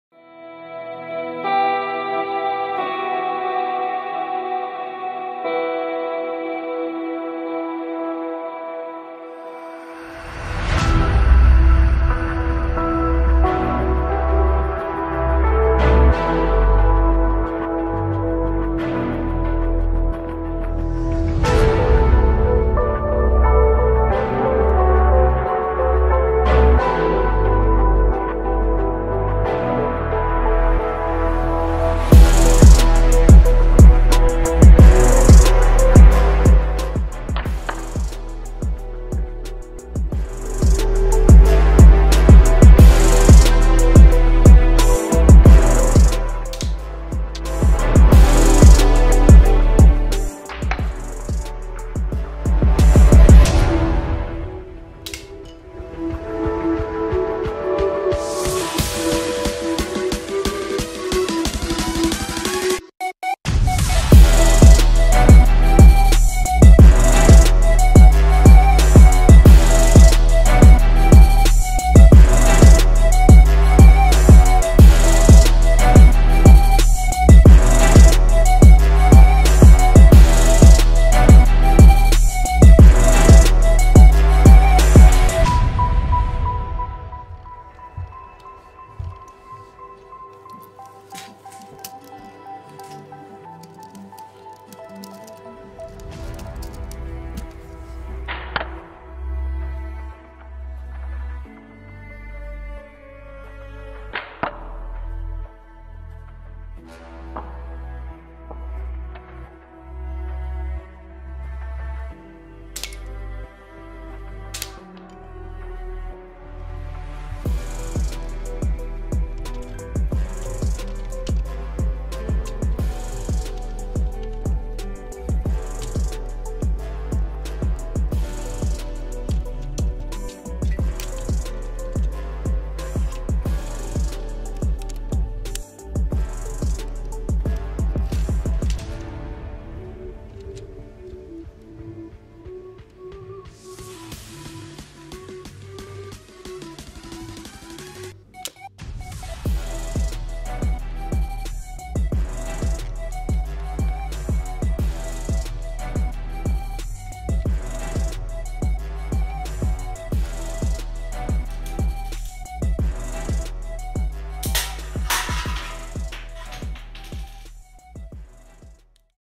Working LEGO Sniper Rifle (Thank You For 100 Subscribers!🥳)
This is my working Sniper Rifle. I'ts pretty powerful.